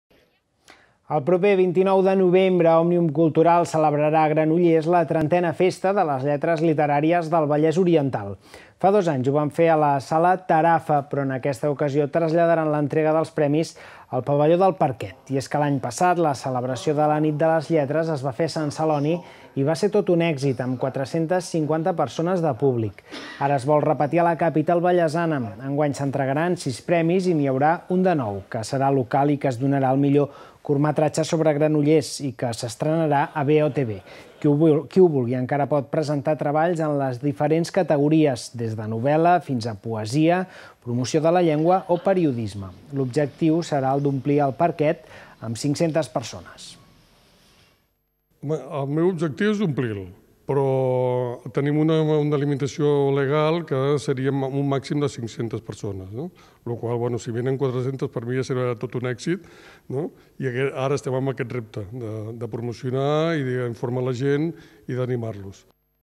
VOTV - Òmnium farà la Nit de les Lletres del Vallès Oriental al Parquet
Notícia emesa a l'informaiu del dia 16 de setembre del 2019.